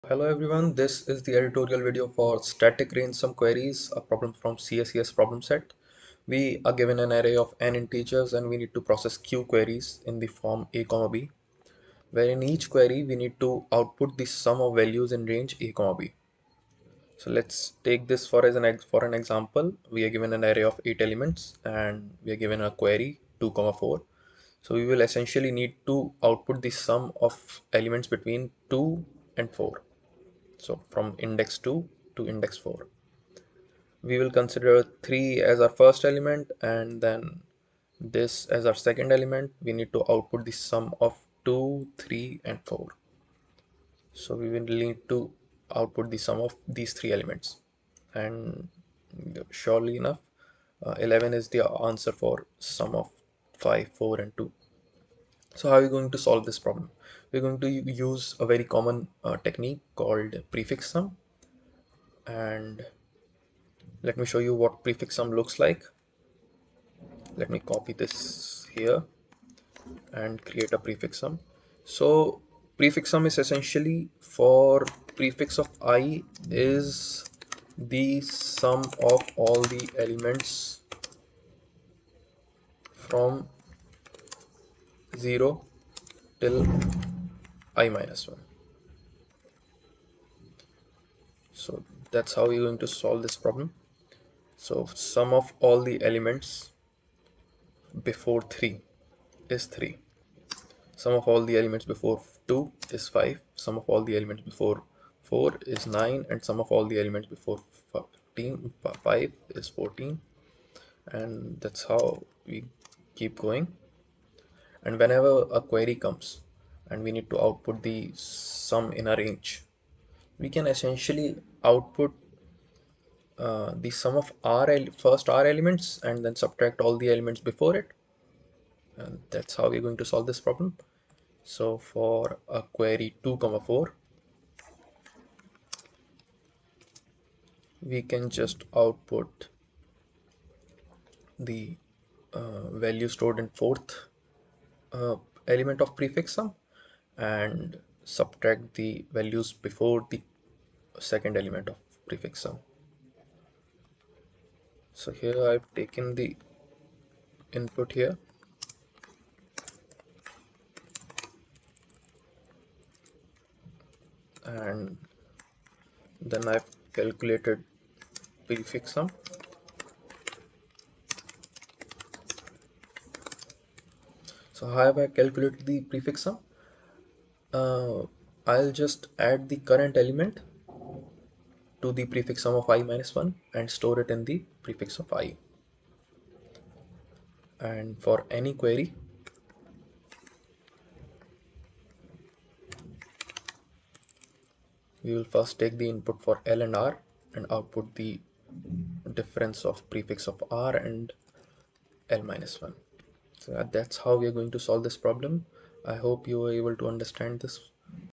01. Static Range Sum Queries (Prefix sum) - CSES
Solution video for Static Range Sum Queries from CSES problem-set.